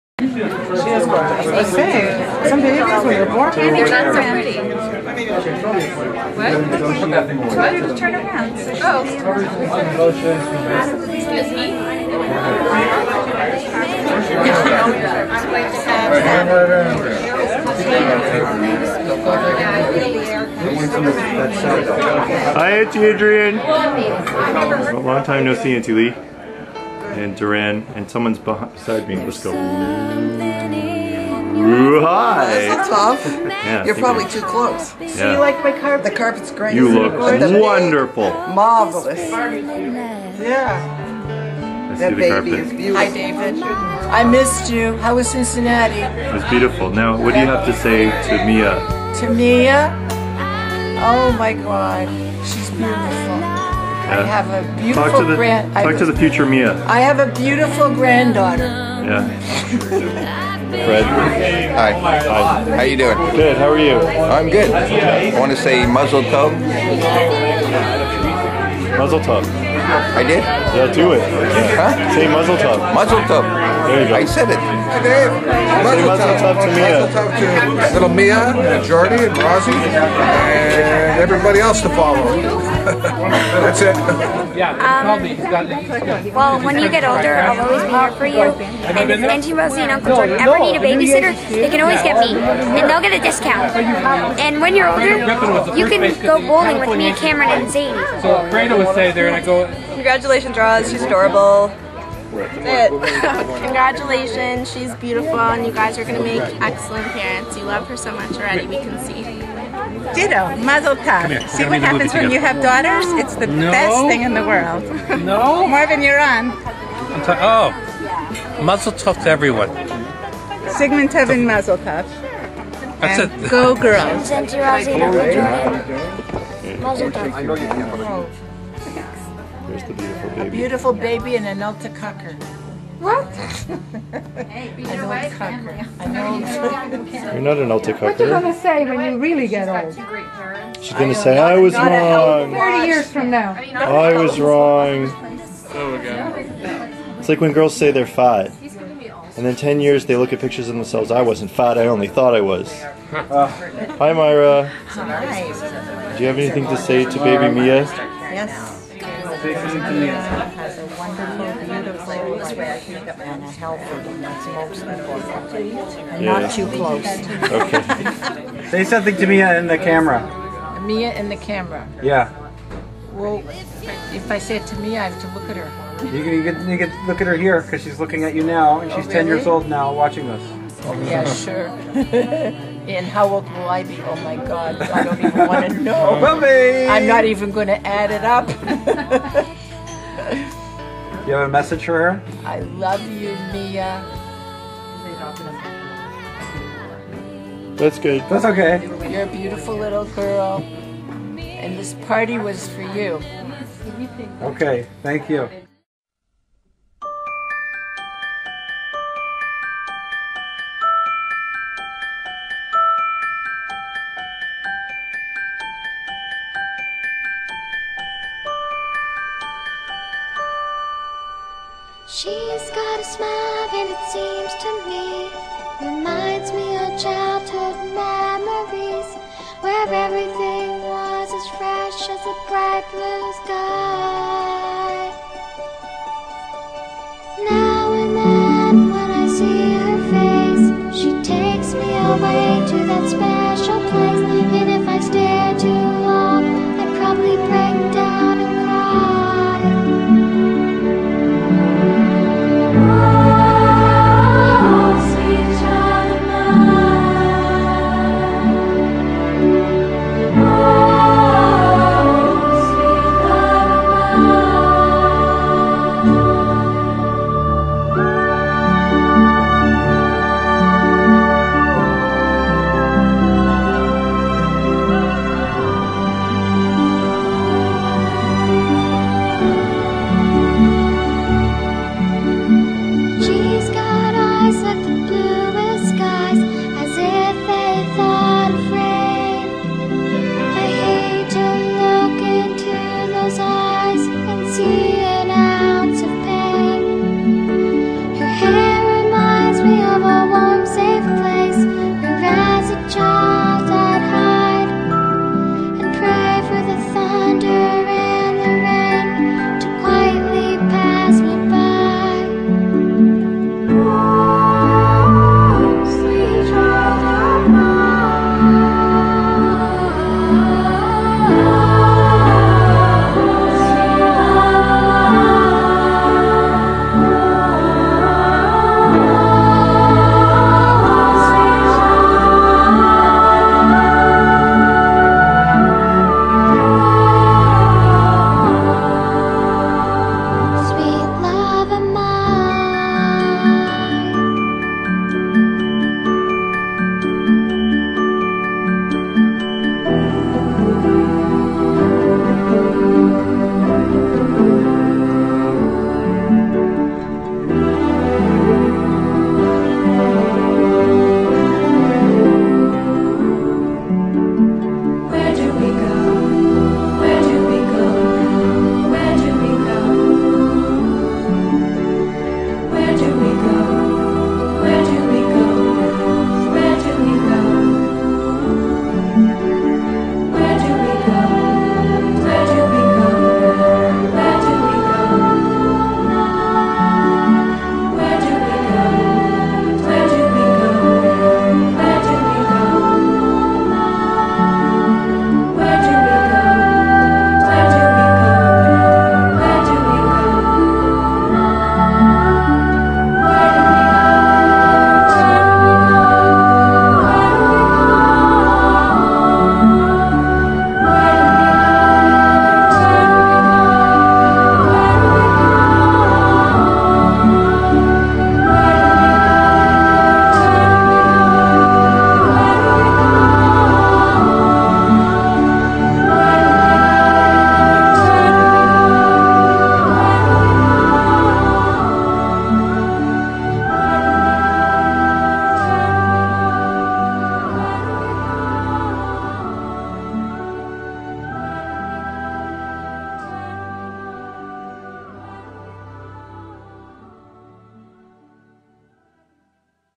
Mia's Baby Naming
This is my brand new niece's baby naming! Jordan and Roz picked gorgeous names - Mia Daphne, which fit this gorgeous baby. Have a look at the photo slideshow half way through the video! Jordan's photos are really breathtaking.